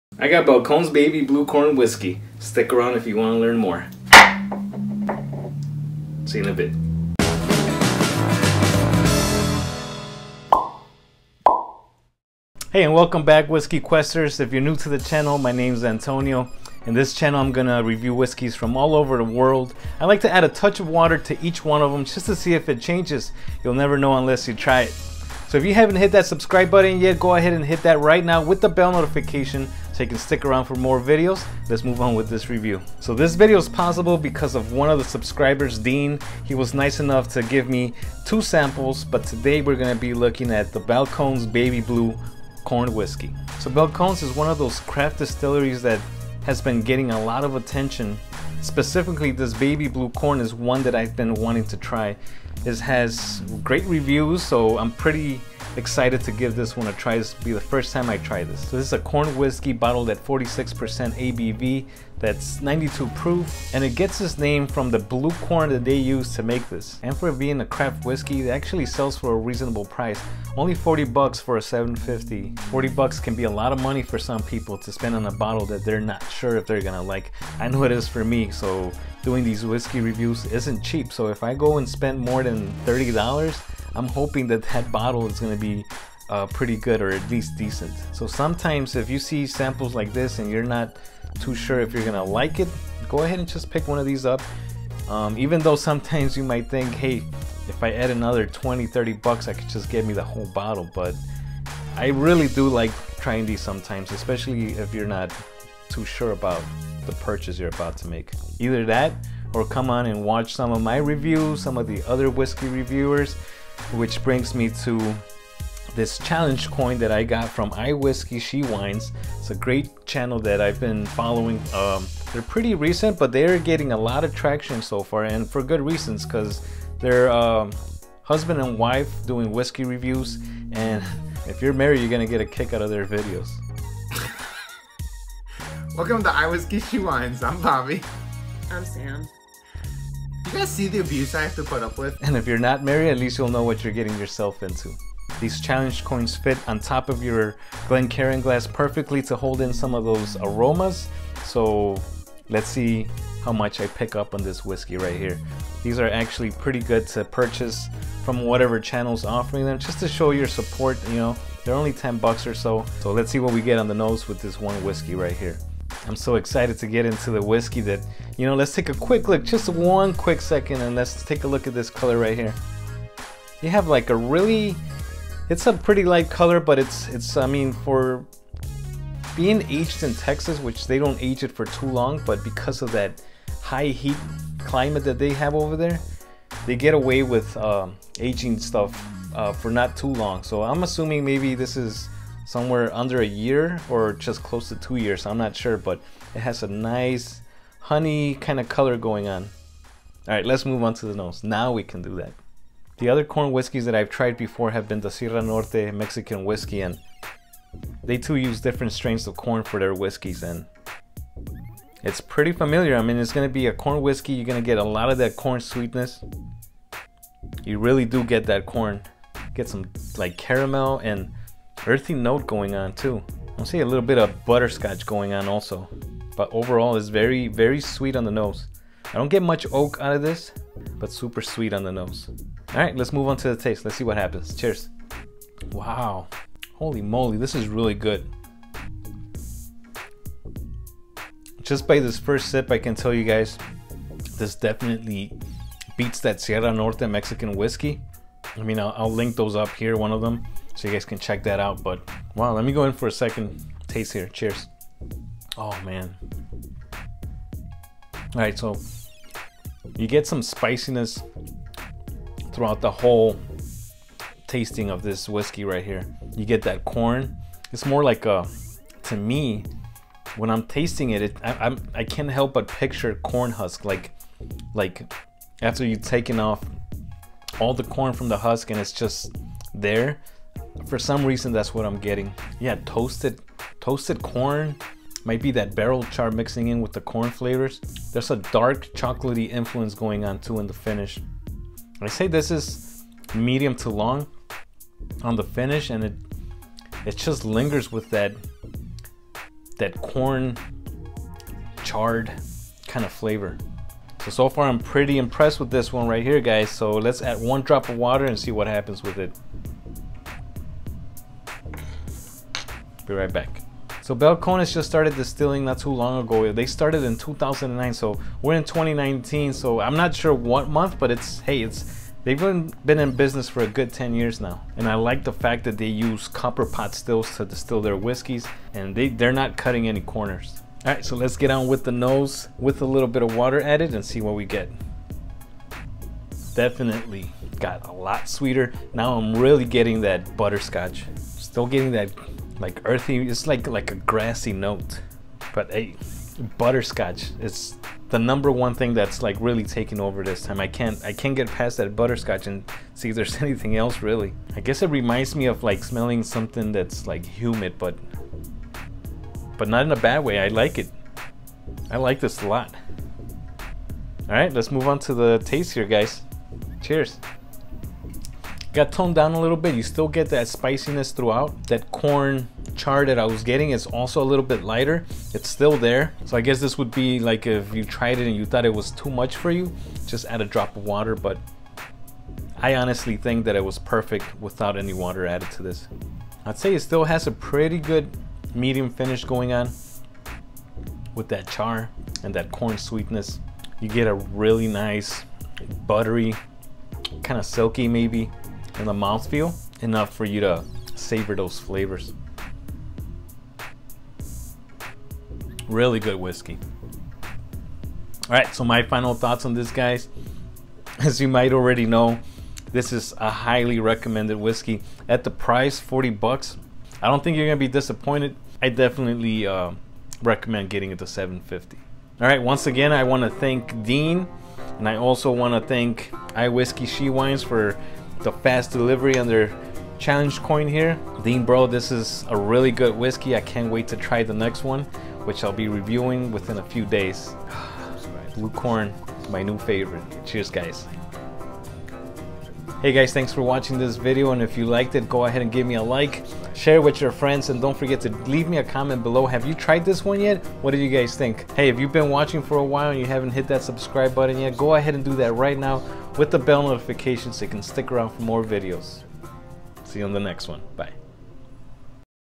Balcones Baby Blue Corn Whiskey Review
In this video I try a sample of Balcones Baby Blue Corn Whiskey. The sample was given to me by one of the subscribers Dean D thank you so much for your support. This is a 40 dollar Texan Corn Whiskey bottled at 92 Proof.

Also go visit I Whiskey She Wines (link below) and tell them Whiskey Quests sent you! Cheers!

You Must be of Drinking Age to View This Content!
Please drink responsibly and don't drink and drive!